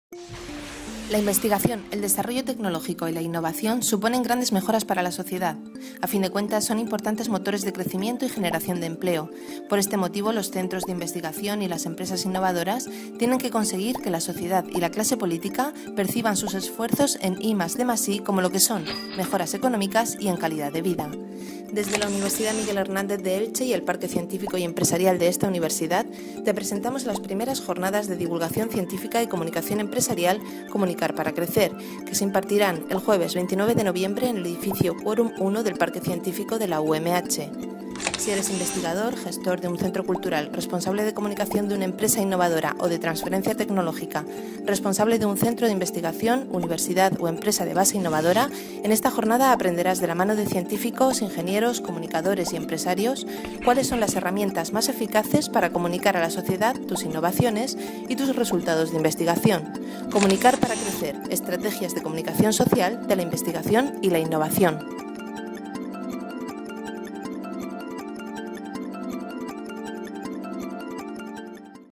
Comunicar para Crecer
Comunicar para Crecer es una Jornada Divulgativa organizada por la UMH y la Fundación Quórum, y dirigida a responsables de cultura, marketing y comunicación en centros de transferencia tecnológica; empresas innovadoras, investigadores ó técnicos de I+D. Más info